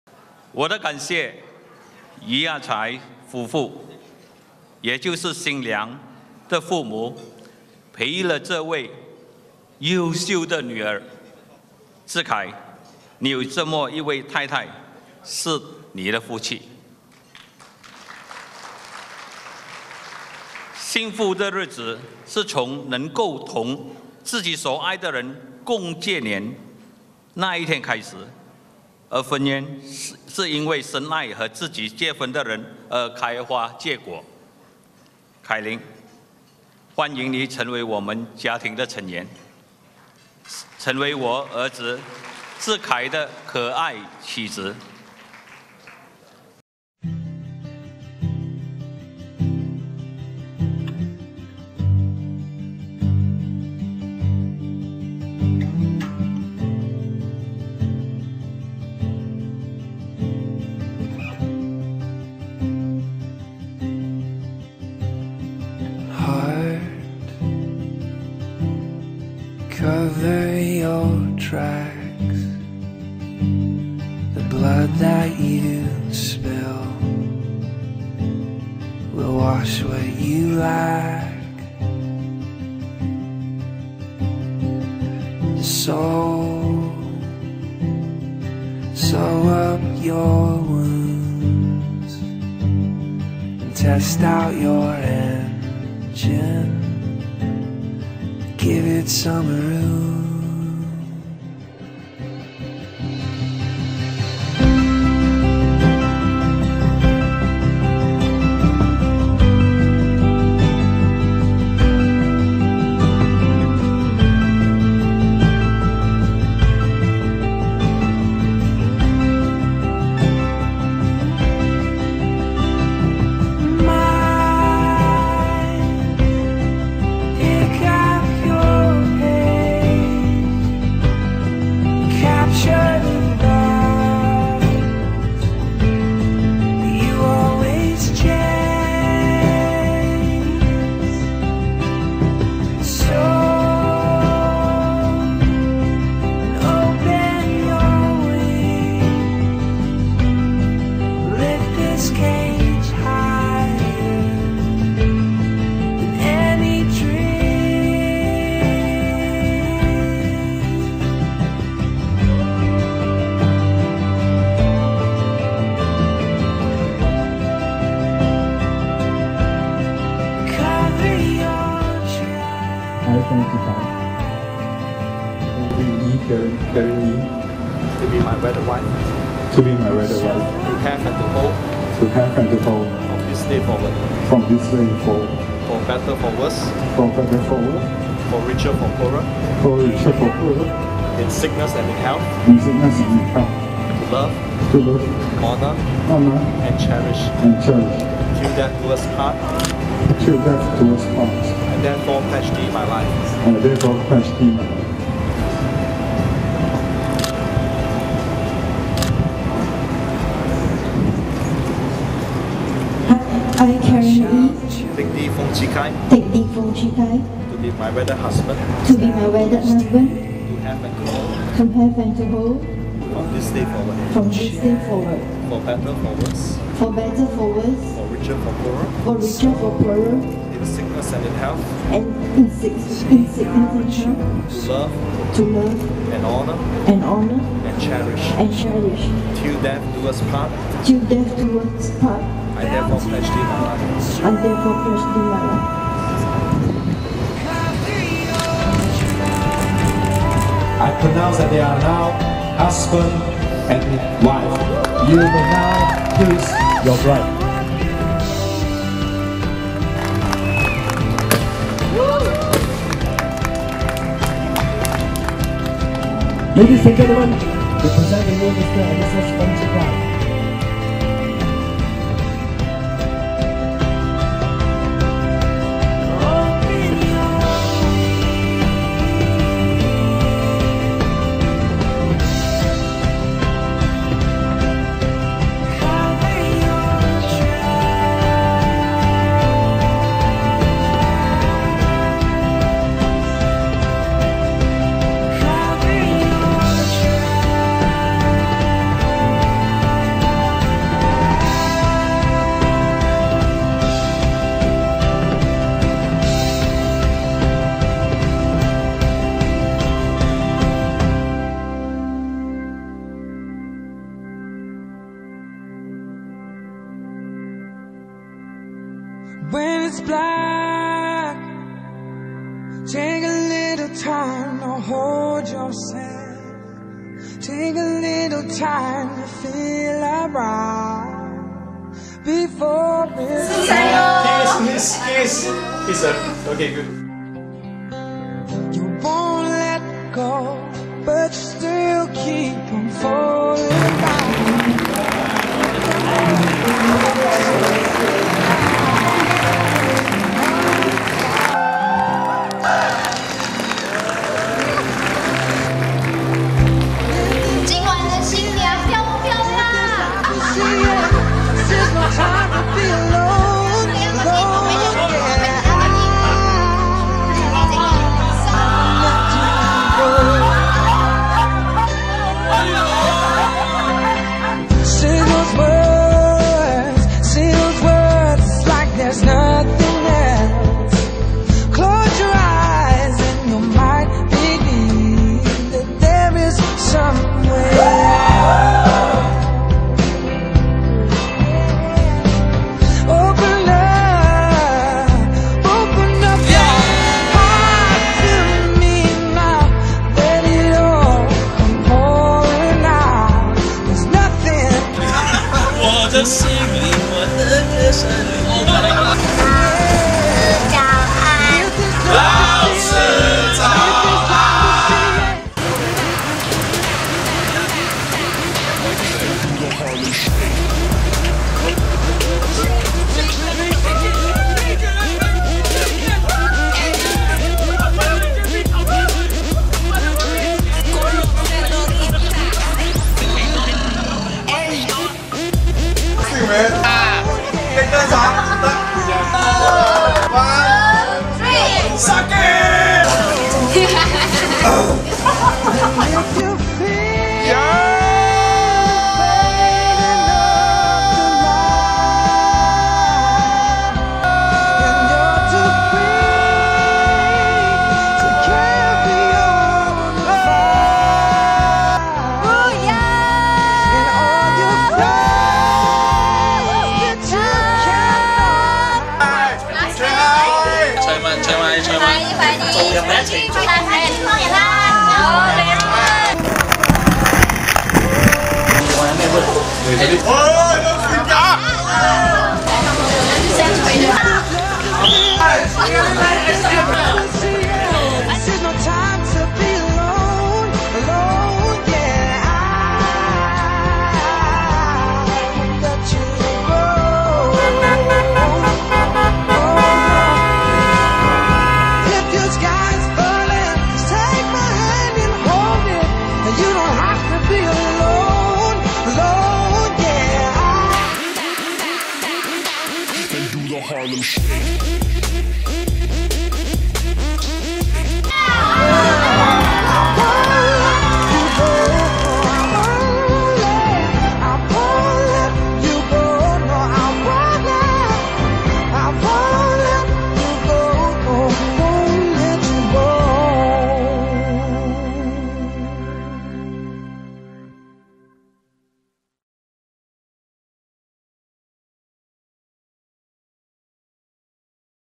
Chee Khai & Caryn Ee // SDE // In Full HD Wedding Highlight Cinema by Brian Heu 0162761519
Lovely Couple wedding, Let's see the wedding Same day edit and Share they Happiness, the wedding of CHEE KHAI & CARYN EE